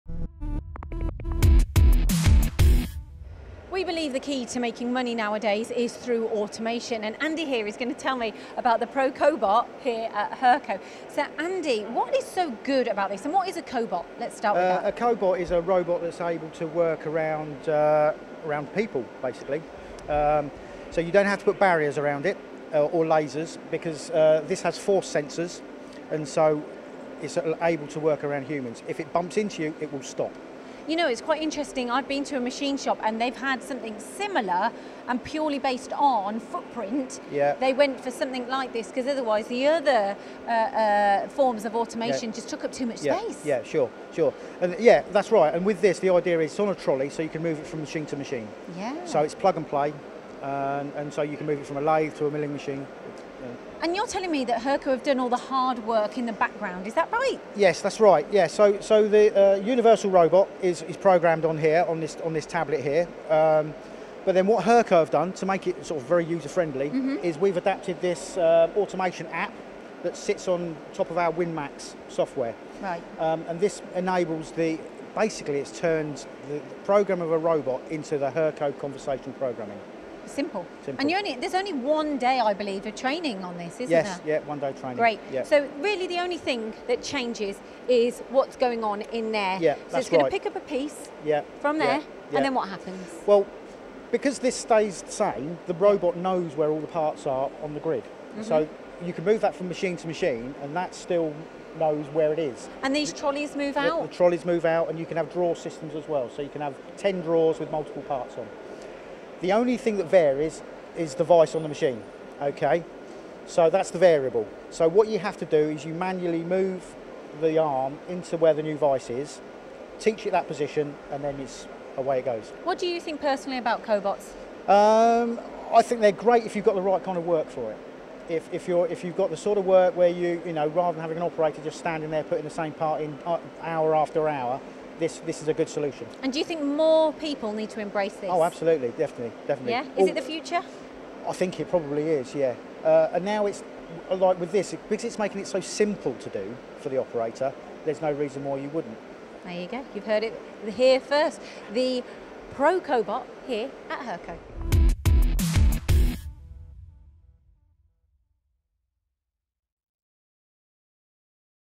Hurco users, time for lights out machining?
Cobots seem to becoming the go to solution for easy to use, low cost production. In this feature Lyndsey is at Hurco taking to Andrew about Hurco's Cobot offering. Working seamlessly with their popular control system, the integration of this unit is simple. Is it time for lights out machining?